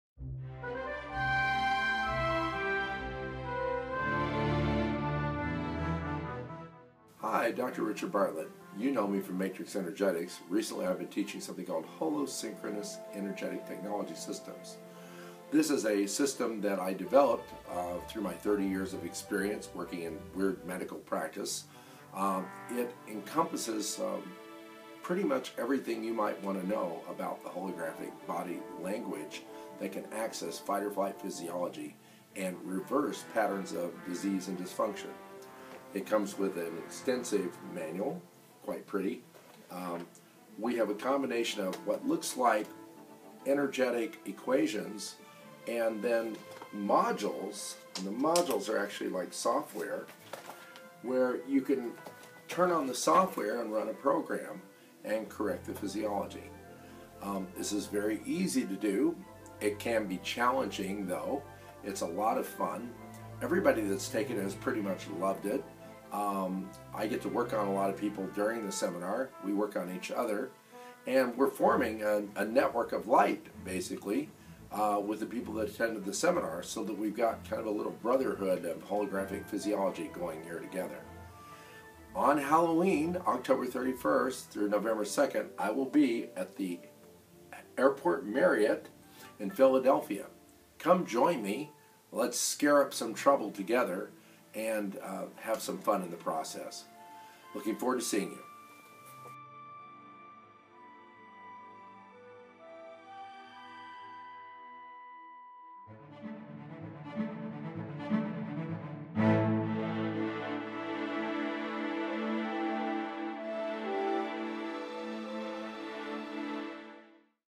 Holo-Synchronous Energetic Technology Systems 10/31-11/02/15
Learn how to reverse patterns of dysfunction through energetic equations and modules! Join Dr. Richard Bartlett for his phenomenal Holo-Synchronous Energetic Technology Systems* Seminar In Philadelphia Halloween - November 2, 2015! Scare up some trouble and have fun in the process; he is looking forward to seeing you!